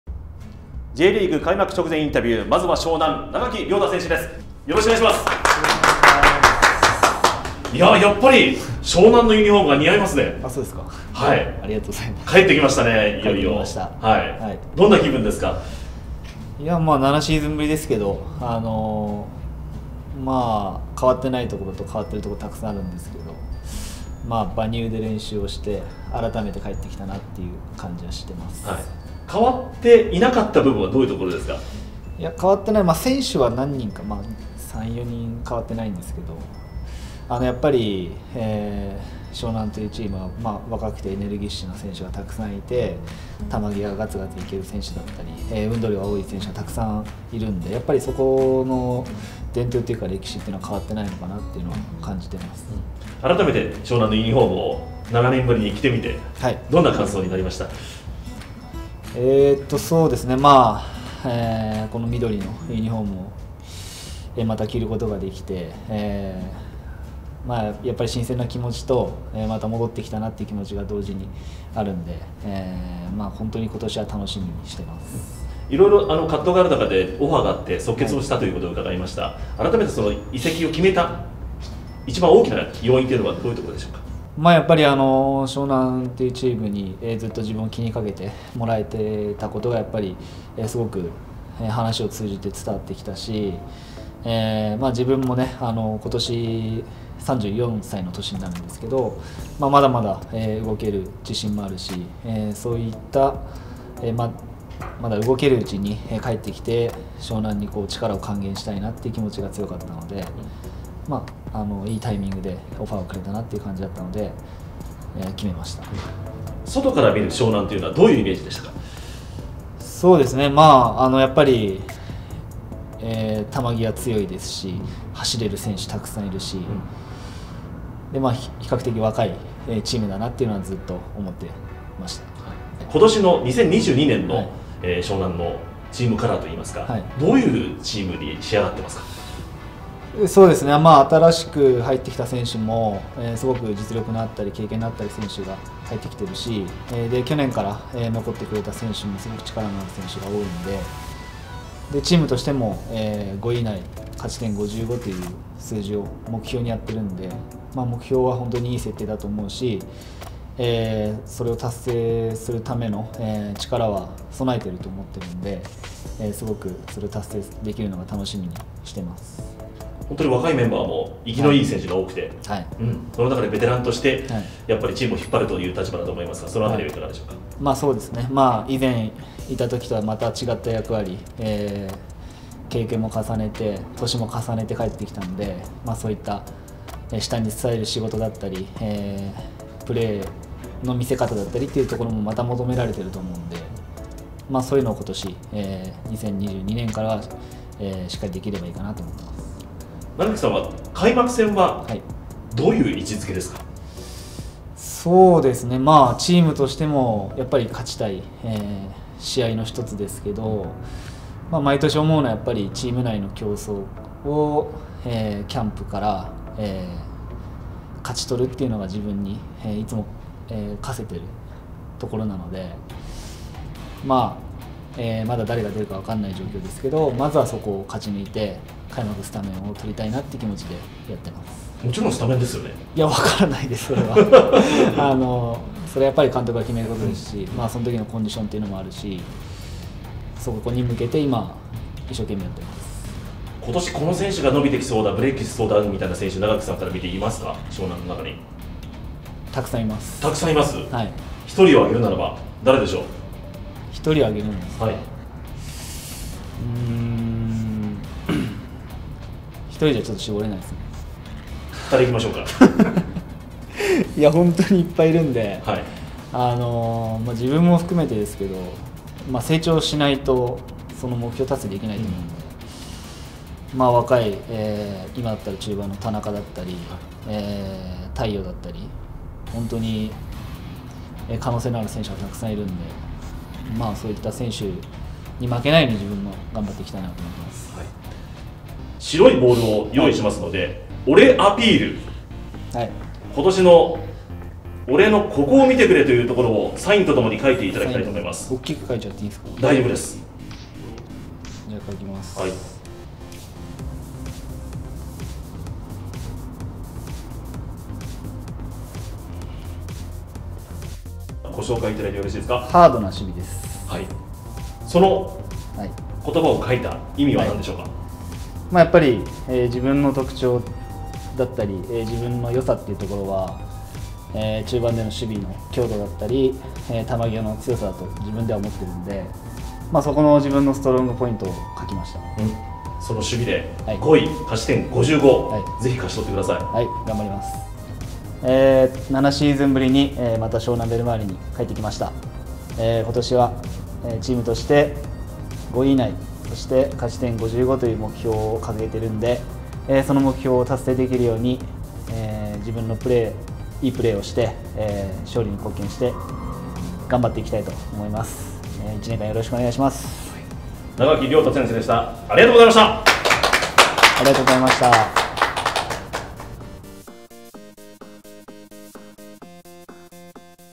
【J開幕直前インタビュー】湘南ベルマーレ永木亮太「俺のここを見ろ！俺アピール」
【湘南ベルマーレ永木亮太選手インタビュー】
2月18日にJリーグが開幕します！
鹿島アントラーズから7年ぶりに湘南へと復帰した
永木亮太選手にインタビュー！

古巣復帰や開幕戦、
湘南の注目若手選手などを語ってもらっています！

さらに、「俺アピール」と称し、
「今シーズンの俺のここを見てほしい！」ポイントを発表！